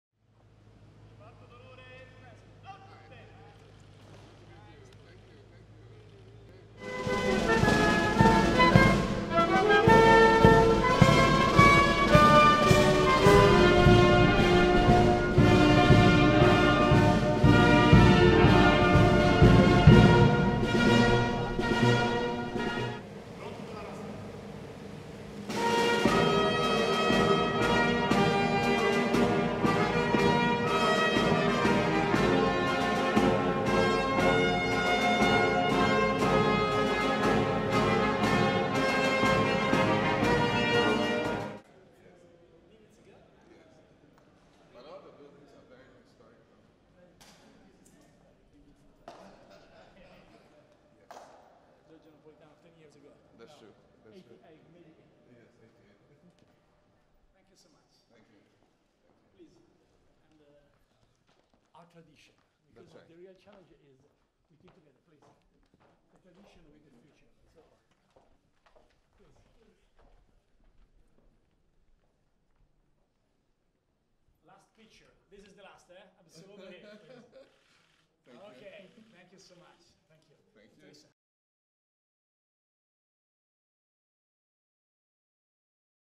Renzi riceve il Presidente della Repubblica del Ghana
Il presidente del Consiglio Matteo Renzi ha incontrato a Palazzo Chigi il presidente della Repubblica del Ghana, John Dramani Mahama.